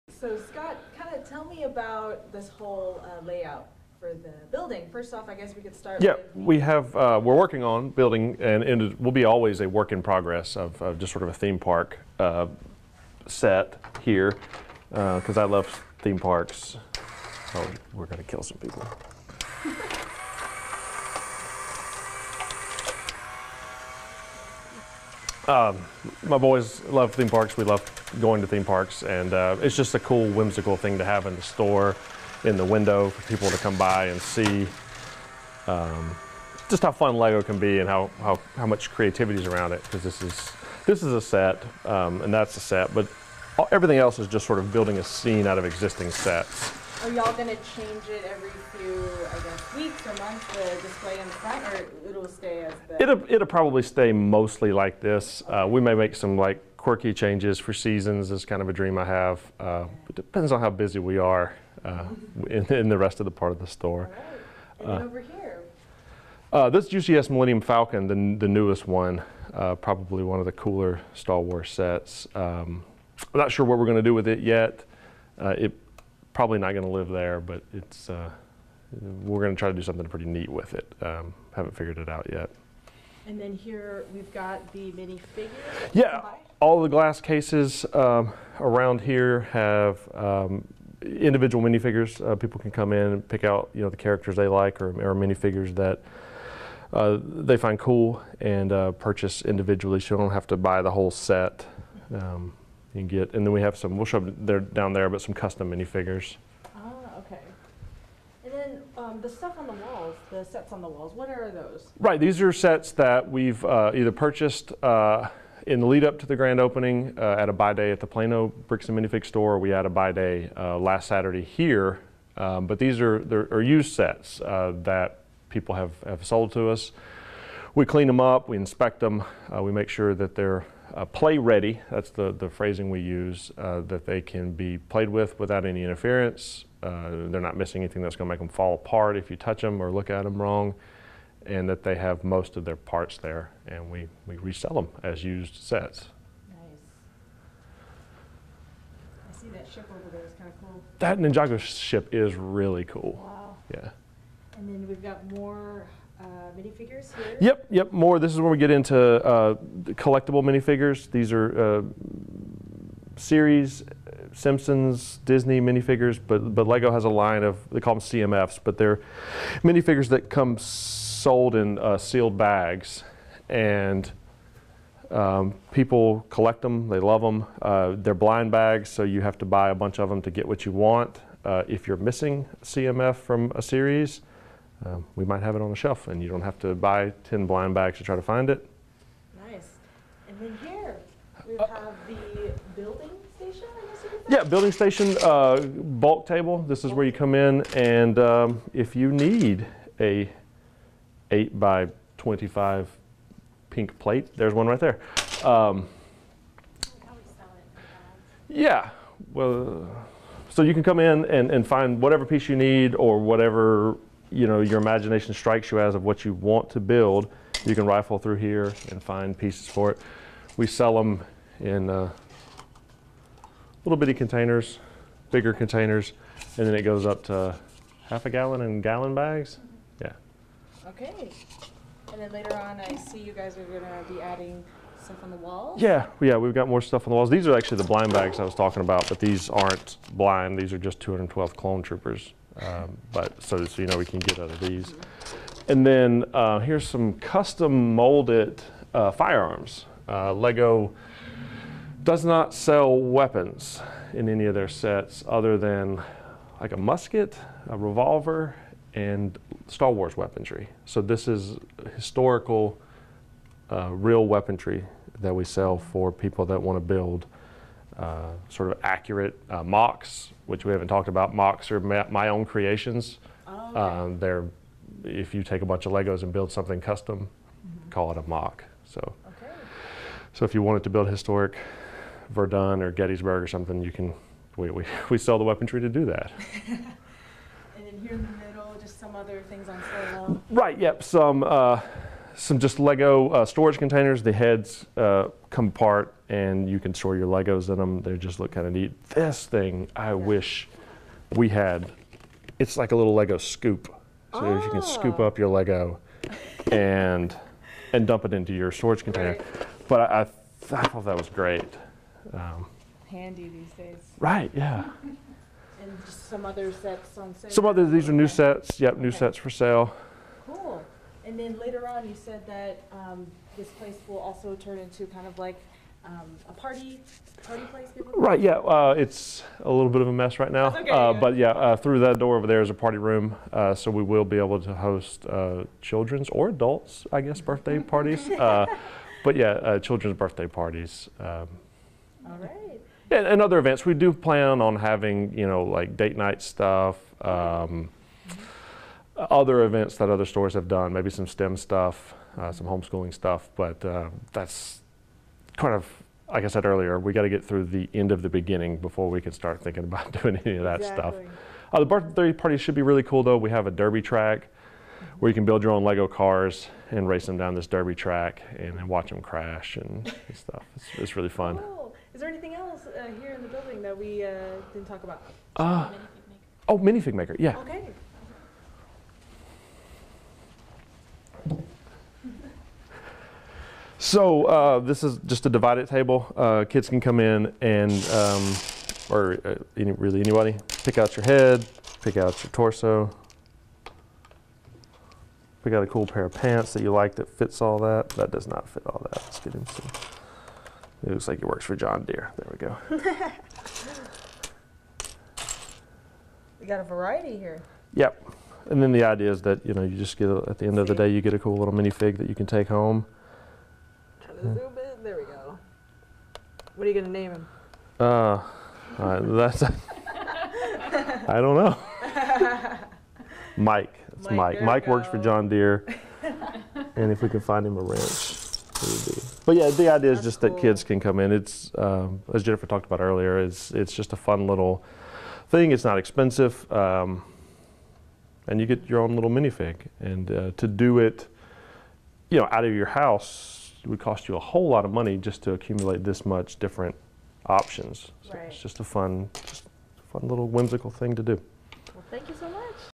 WebXtra: Bricks and Minifigs LEGO store to open Tyler location in June
A new store that specializes in all things LEGO is coming to Tyler on June 17.